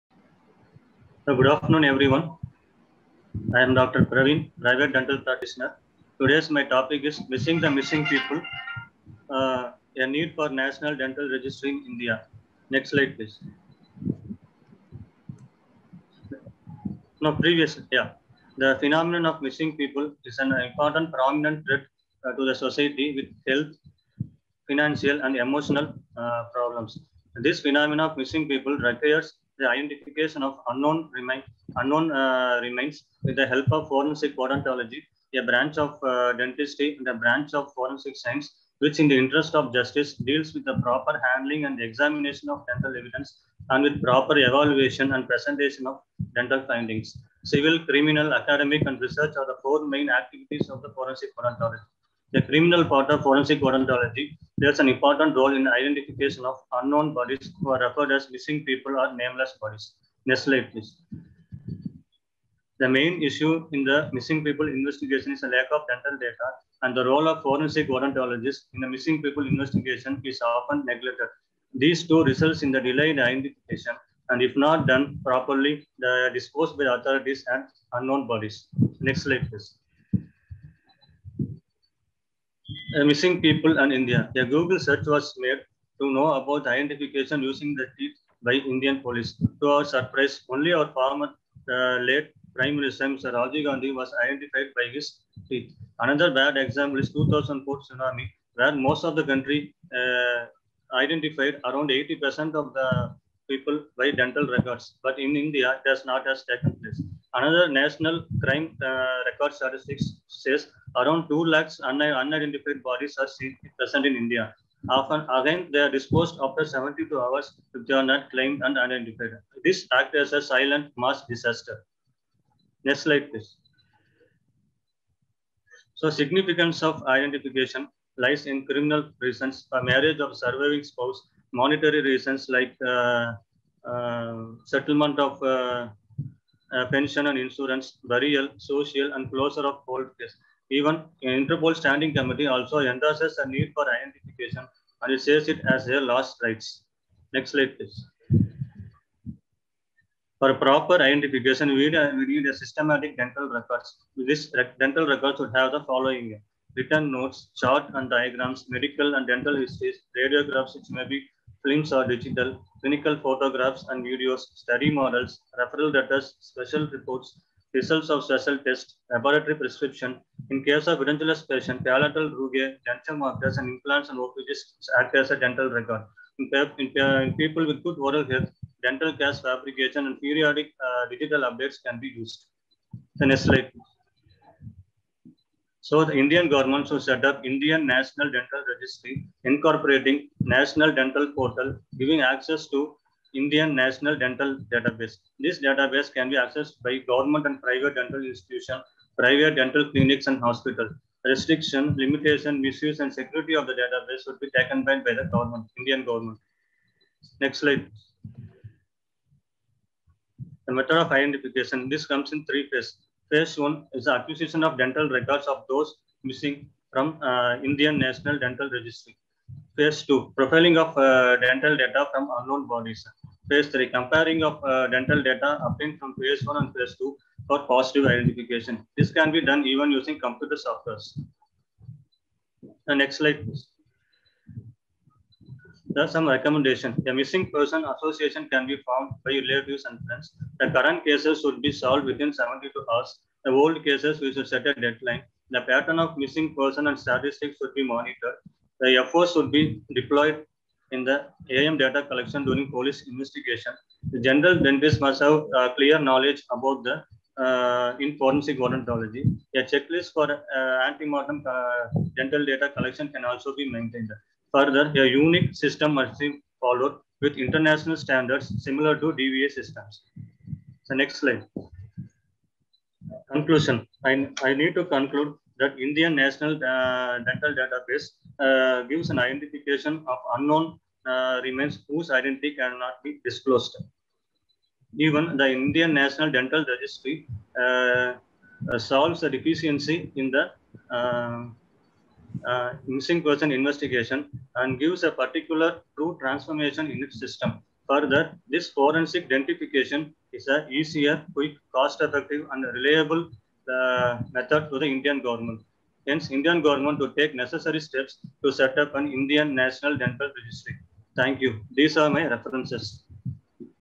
Missing the Missing People- A Need for National Dental Registry (Database) in India:Paper13
Missing people phenomena is of prominent treat to the society with health, emotional and
financial issues. In this phenomena lies also the recovered unidentified human remains, which
should be processed also considering “Forensic Odontology”, a branch of dentistry and
forensic sciences, which deals, in the interest of justice, with handling, examination and
evaluation of dental evidence and dental findings. Civil, criminal and research are the triad of
forensic odontology. Forensic odontology deals also with identification of unknown bodies
who are referred as “missing persons and nameless bodies”. The main issue in the
identification of human remains and the collection of identifying data is the availability of
dental data of the missing persons by Dr. S Praveen.